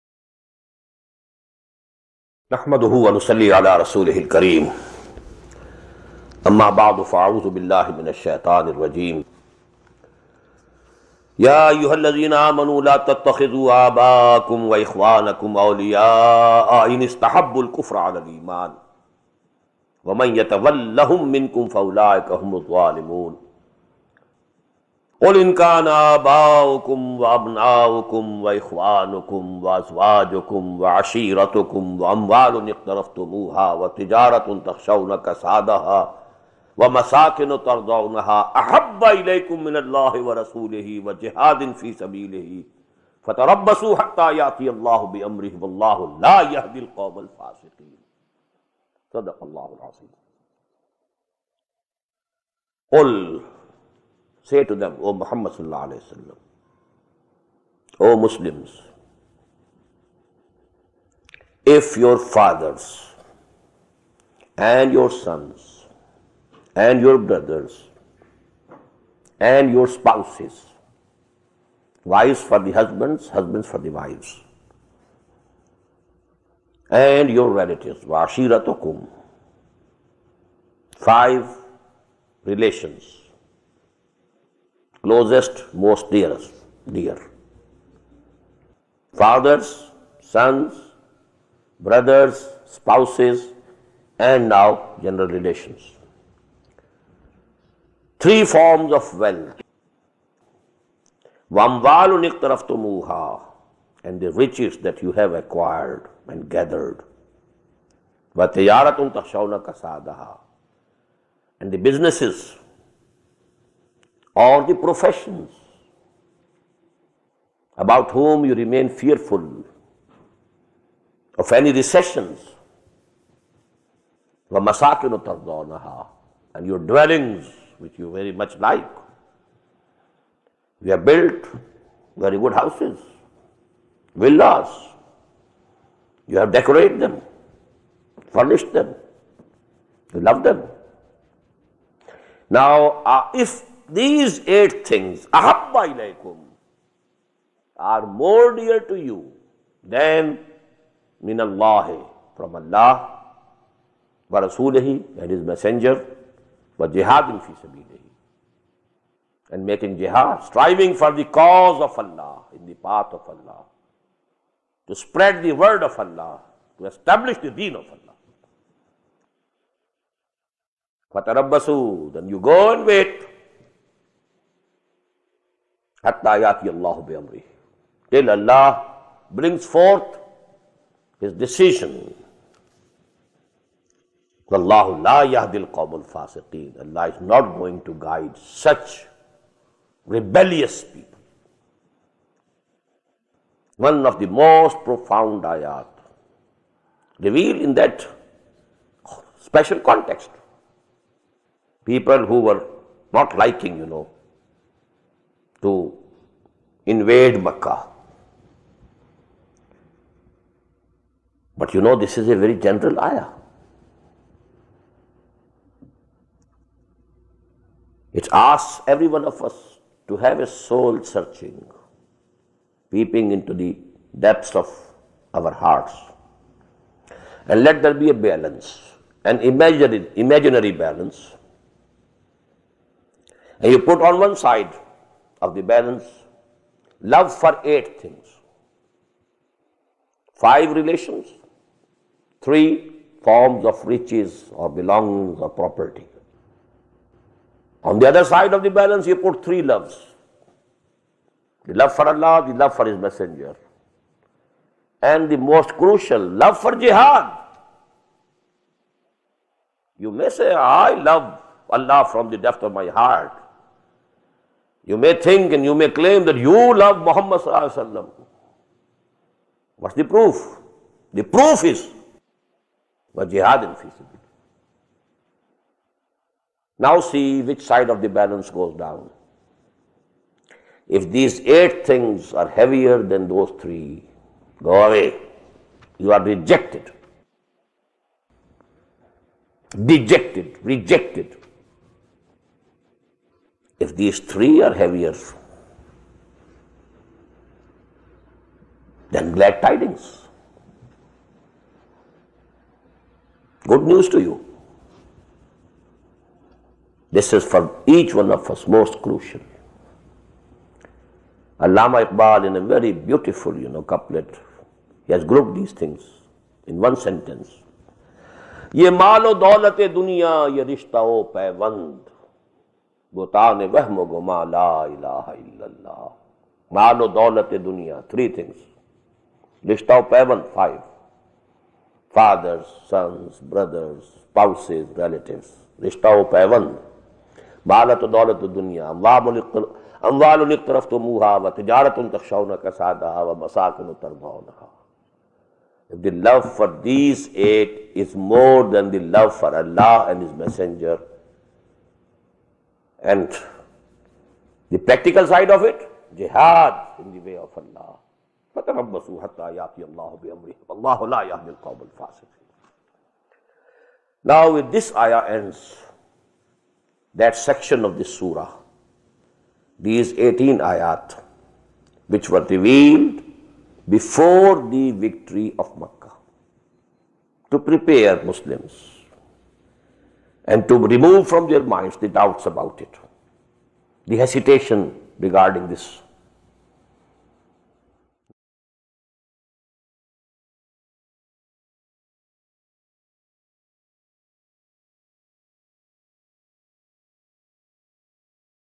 Surah Taubah Ayah 24 Tafseer in English by Dr Israr Ahmed
Bayan ul Huda, Surah Taubah Ayah 24 Tafseer in English by Dr Israr Ahmed

TOPICS COVERED IN THIS VIDEO:
1. If you love your fathers, sons, spouses, wealth, and worldly life more than ALLAH, His Rasul ﷺ, and Jihad, then go and wait, since ALLAH does not guide such rebellious people.

Narrated `Aisha:
Whenever the Prophet (ﷺ) took the bath of Janaba (sexual relation or wet dream) he asked for the Hilab or some other scent. He used to take it in his hand, rub it first over the right side of his head and then over the left and then rub the middle of his head with both hands.